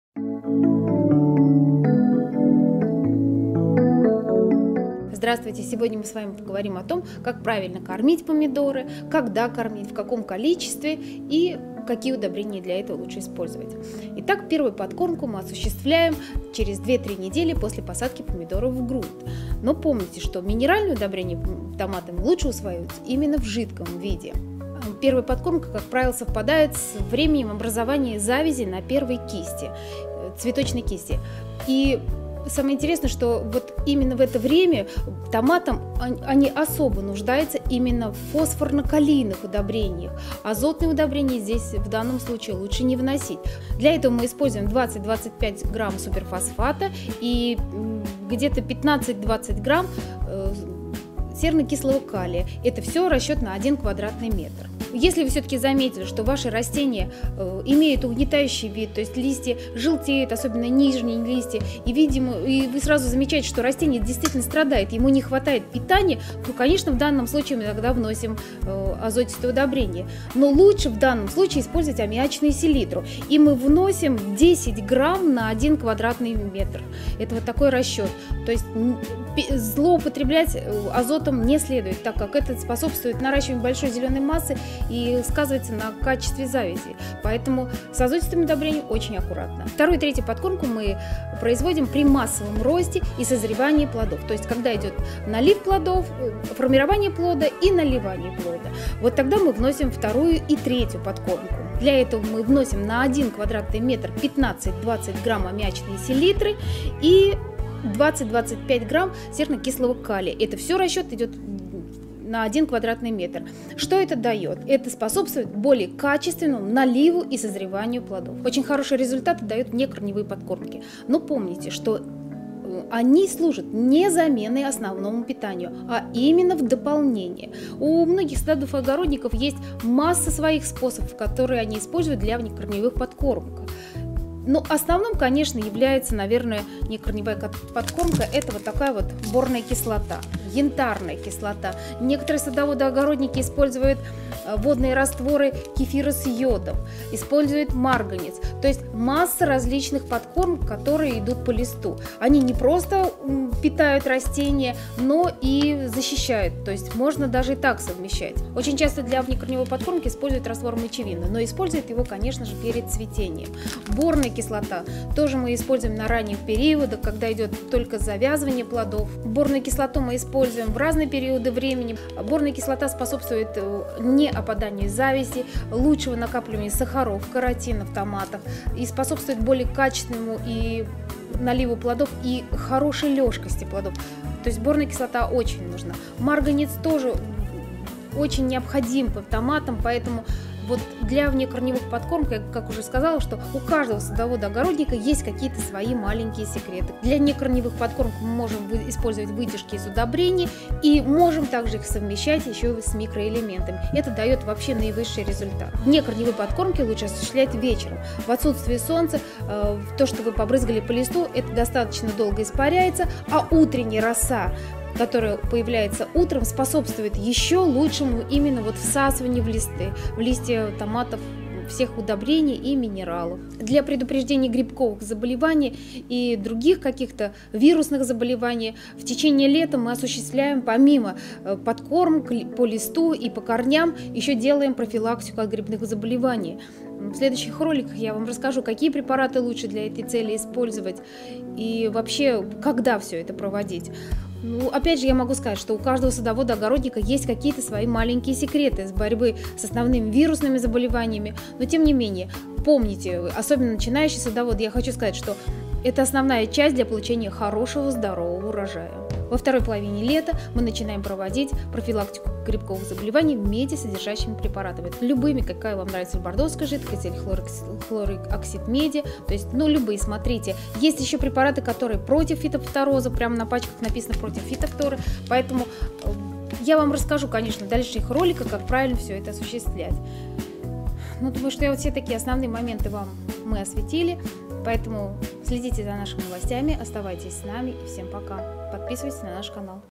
Подкормка томатов 🍅 Сроки внесения ✅ Какие подкормки использовать? Сад и огород с HitsadTV
Подкормка томатов 🍅 Сроки внесения ✅ Какие подкормки использовать?
Расскажем чем и когда подкормить томаты. Чем полезна для помидор внекорневая подкормка:
✅ Янтарная кислота
✅ Азотные удобрения
✅ Борная кислота
✅ Бордоская смесь

Как когда и в каком количестве правильно кормить помидоры, и какие же удобрения лучше использовать? Первую подкормку нужно осуществлять через две  недели после посадки помидоры в грунт.
Первая подкормка совпадает с временем образования завязей на первой кисти.  Для подкормки необходимо использовать 20 – 25 грамм суперфосфата и 15 -20 грамм сернокислого калия.  Если же Вы все-таки заметили, что растения имеют угнетающий вид, растение страдает и ему не хватает питание то тогда, конечно же, можно внести азотистое удобрение но лучше использовать аммиачную селитру.

Вносить аммиачную селитру нужно 10 грамм на один квадратный метр. Вторую и третью подкормку необходимо производить при массовом росте и созревании плодов. Для этого необходимо внести на один квадратный метр 15 -20 грамм аммиачной селитры и 20 -25 грамм сернокислого калия. Это будет способствовать более качественному наливу и созреванию плодов. Так же очень хорошие результаты дают внекорневые подкормки, но помните, что они служат не заменой основному питанию, а именно дополнением к нему. Основной внекорневой подкормкой является «Борная кислота» и «Янтарная кислота».

«Борная кислота» используется на раннем периоде, когда идет только завязывание плодов, использовать ее можно в разные периоды времени. Она способствует не опаданию завязей, более качественному наливу плодов. Для внекорневой подкормки можно так же использовать выдержки из удобрений и может так же их совмещать с микроэлементами. Внекорневые подкорми лучше осуществлять вечером в отсутствии солнца.

Сад и огород с HitsadTV

Выращивание овощей из семян - выбираем грунт для рассады.